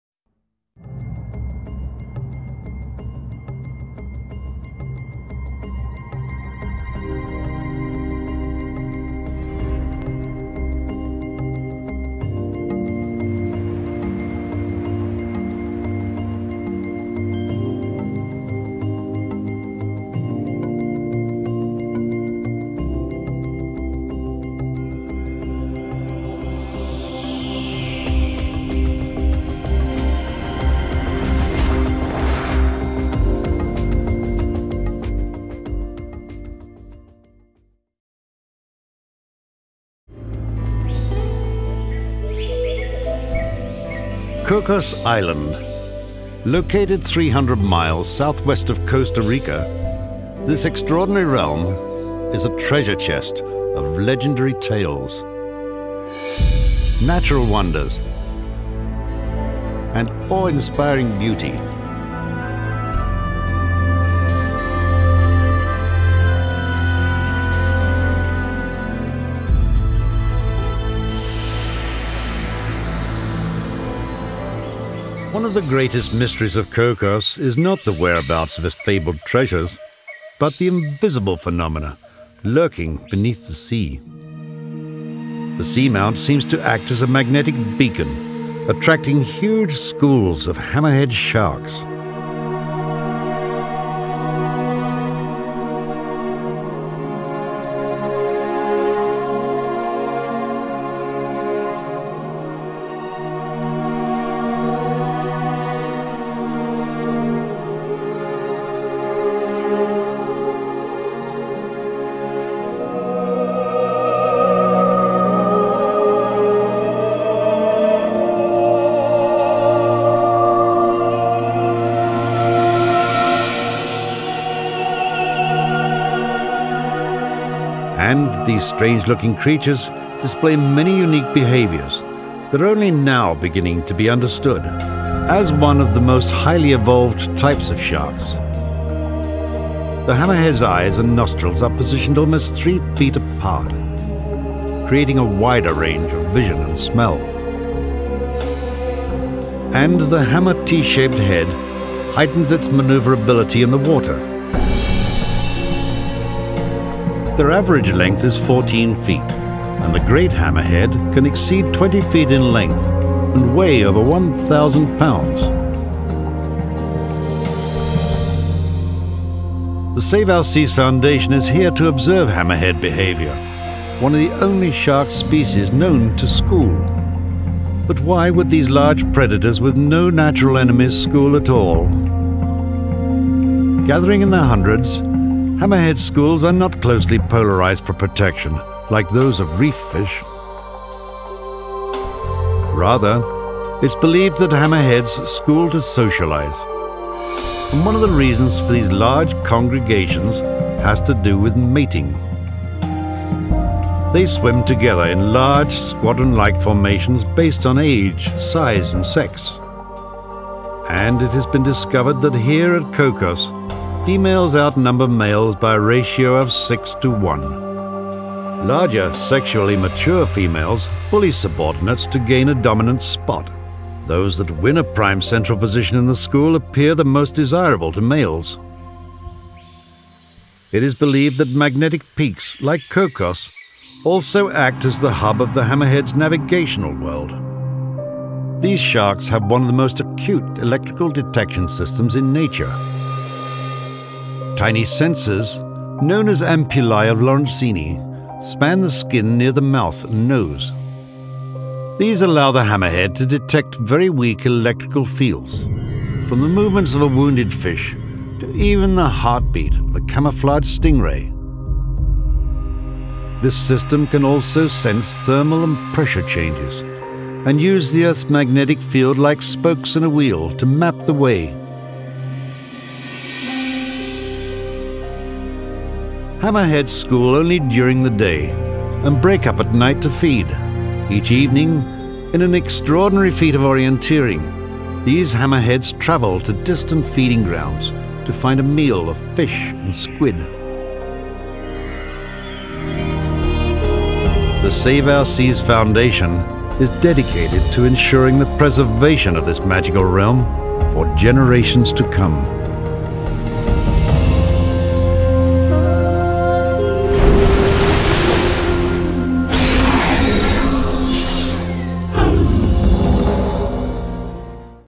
6: Cocos Island - Part Two
Cocos Island attracts large numbers of hammerhead sharks every year. In our second episode on Cocos we take a closer look at these fascinating sharks. Hammerheads are one of the only known species of sharks to school. There are nine known species of hammerhead including the Great hammerhead and the Scalloped hammerhead which are featured in this show. Join us again in two weeks for the story of Maxine, the ragged-tooth shark.